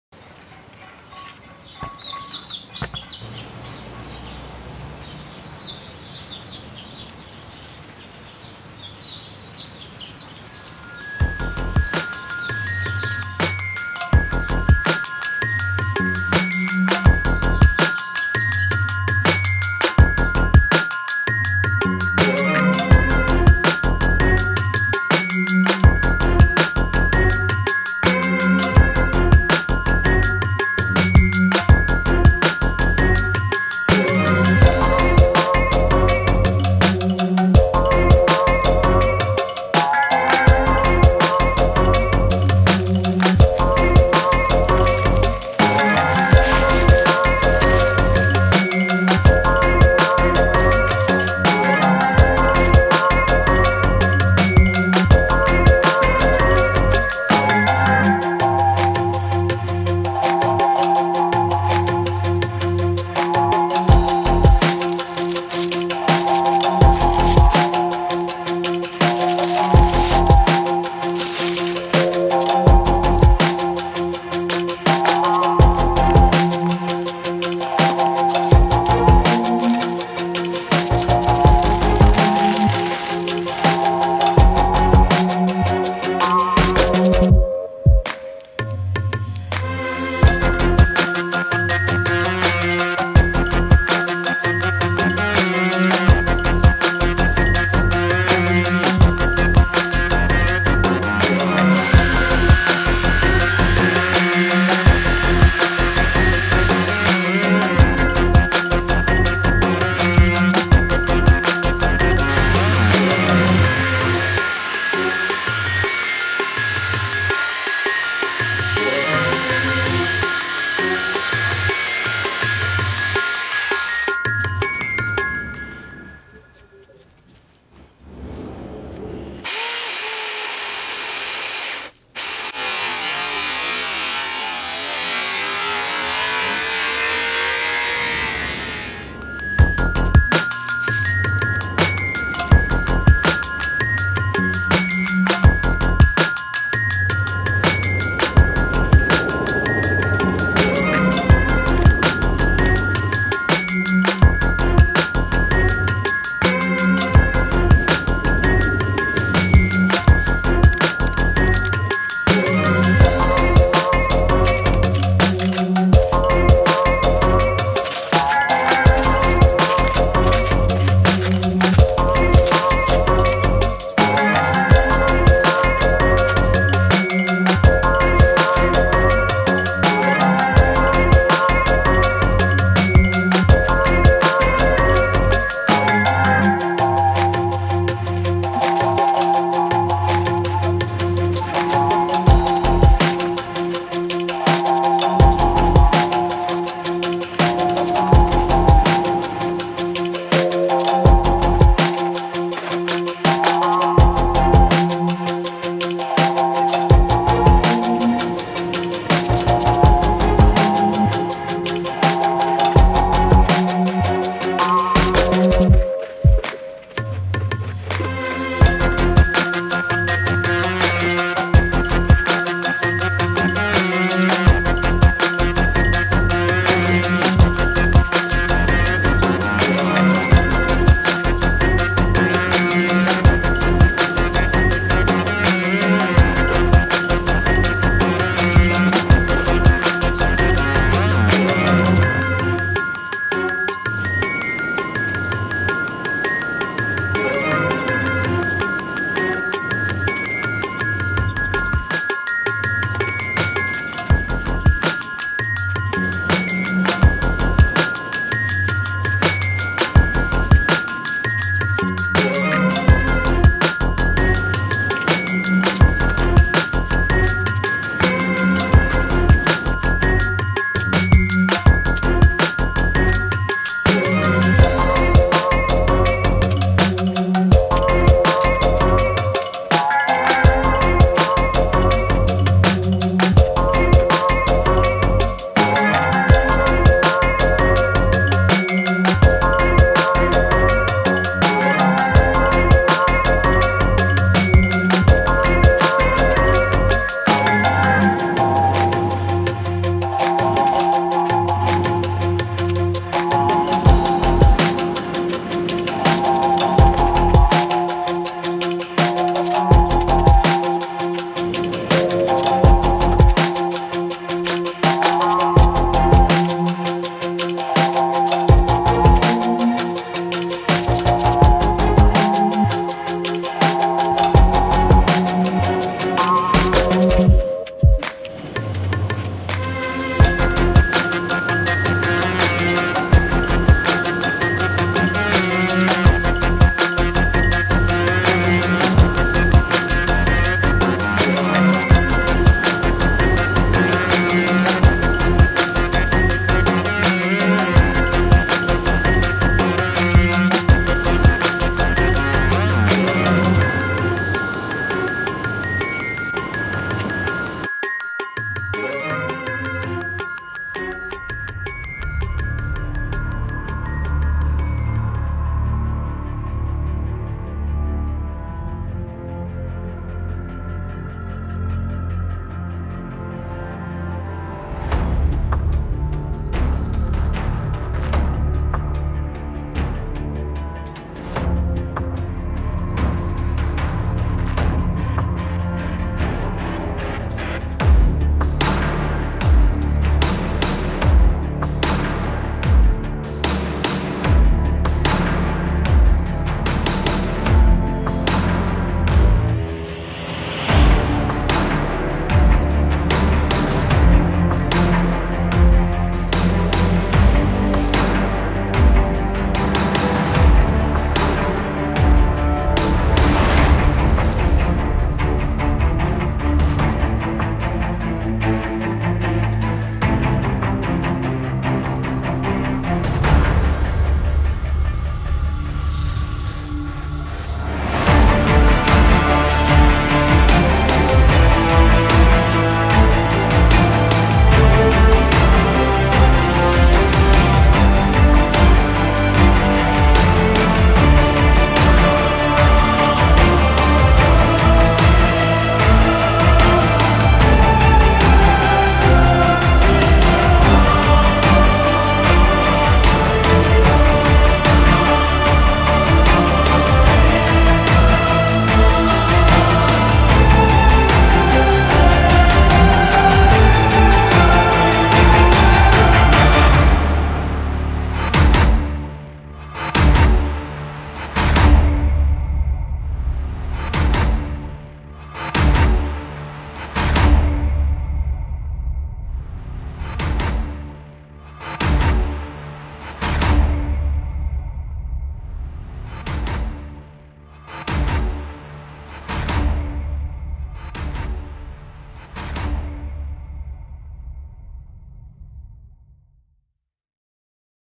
Acerola (retirada dos Alporques) vídeo 3
chega o dia de retirar os alporques da Acerola, já se passaram 3 meses, sem enraizador, somente areia de compostagem e vamos conferir os resultados agora.